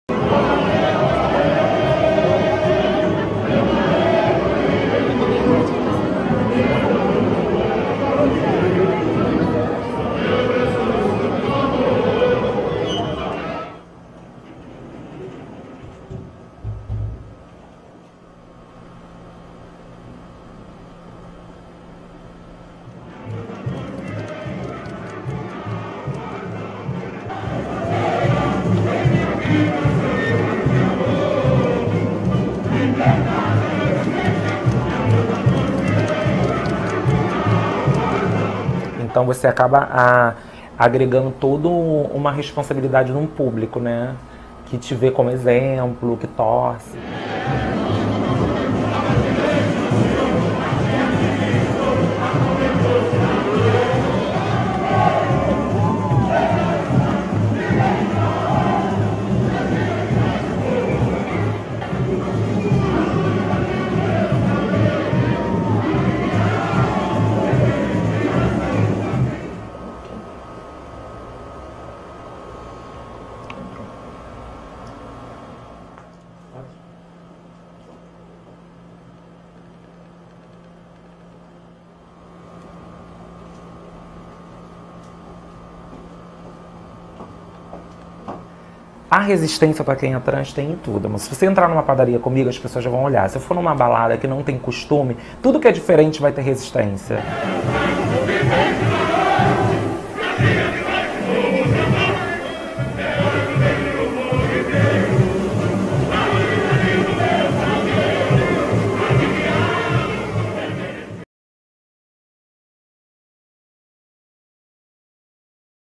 Estrella transgénero del Carnaval de Río abre las puertas a la diversidad. | EL TIEMPO
Kamilla Carvalho, después de algunas cirugías y un largo proceso de redescubrimiento, entrará a la historia como la primera mujer transgénero de una de las principales escuelas de samba de Río de Janeiro y, según la agrupación, la primera de todo el Carnaval carioca.